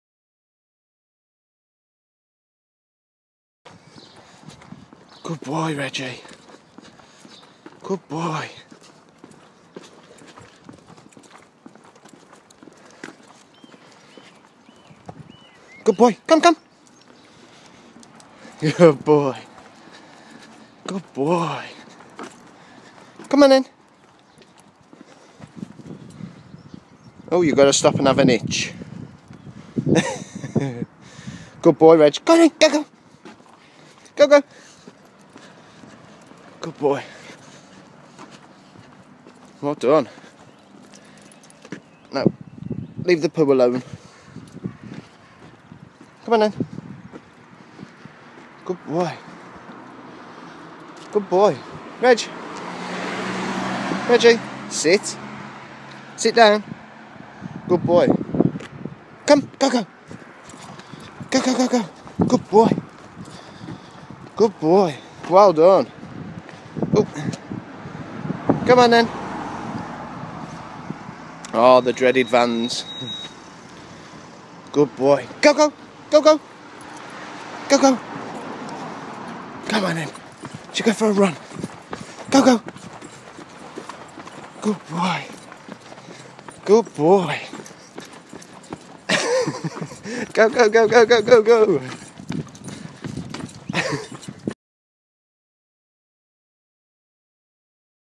Alaskan malamute lead training getting better
Daddy is so proud